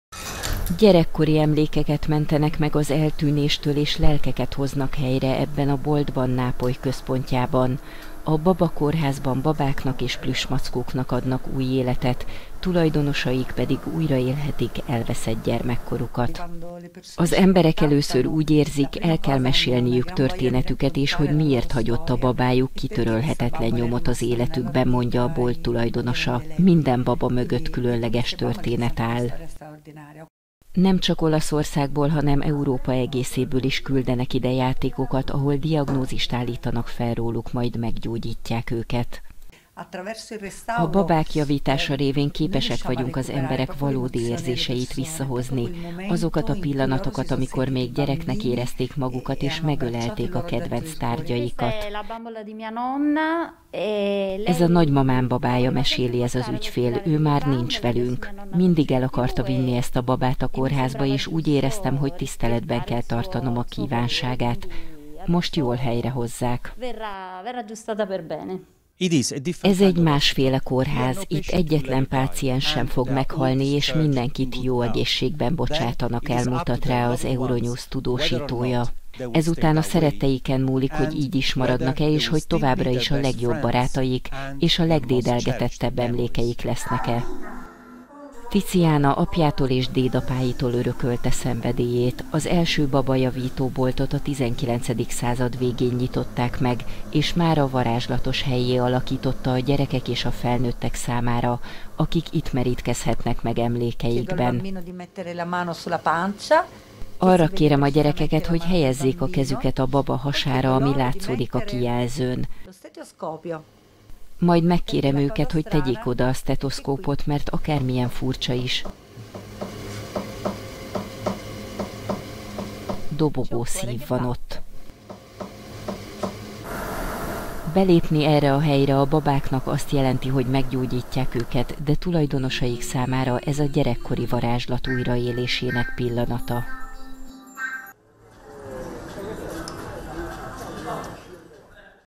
Ahol az emlékek visszatérnek - babakórház Nápolyban
Tiziana apjától és dédapáitól örökölte szenvedélyét. Az első babajavító boltot a 19. század végén nyitották meg, és mára varázslatos hellyé alakította a gyerekek és a felnőttek számára, akik itt merítkezhetnek meg emlékeikben.